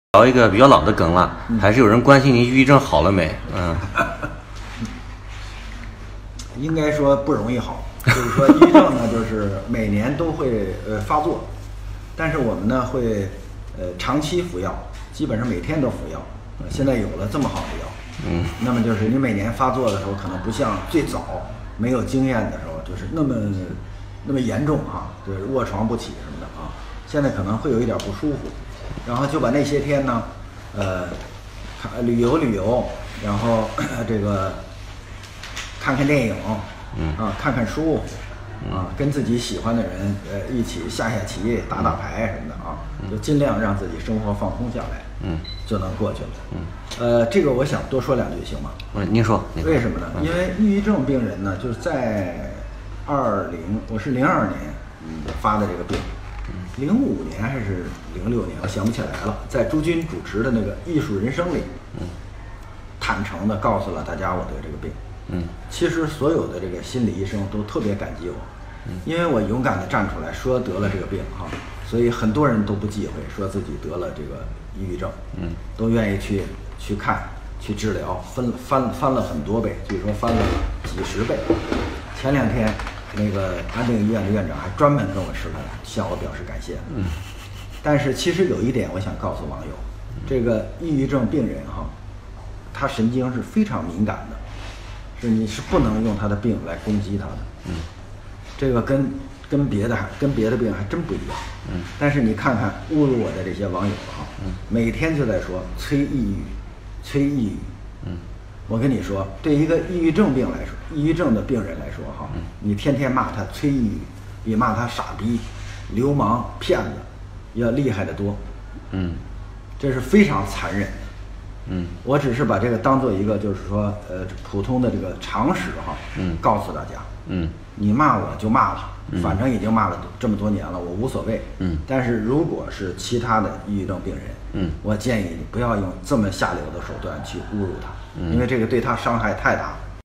谈抑郁症
抑郁症人群神经比较敏感，你是不能用他的病去攻击他的，你骂我就骂了，我无所谓，但如果是其他的抑郁症病人，我建议你不要用这么下流的手段去侮辱他，因为这个对他伤害太大了。